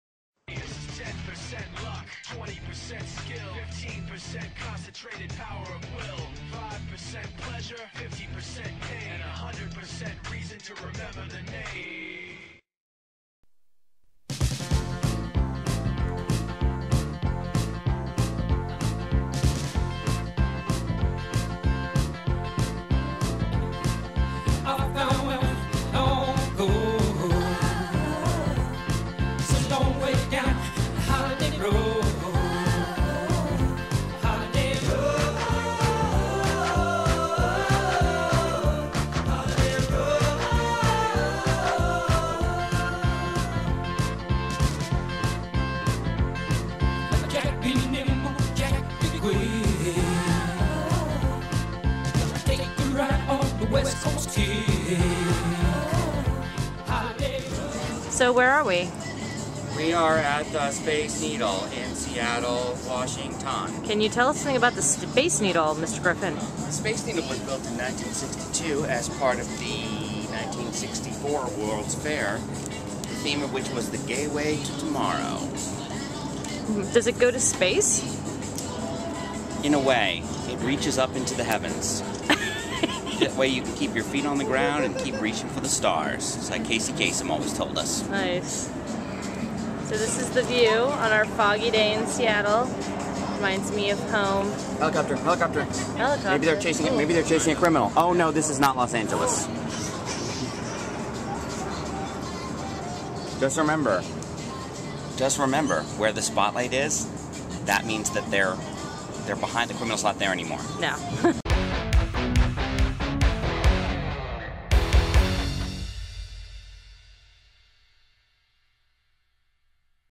Monday Goes to Space
Yeah, I suck. But please enjoy this lovely footage from the Space Needle!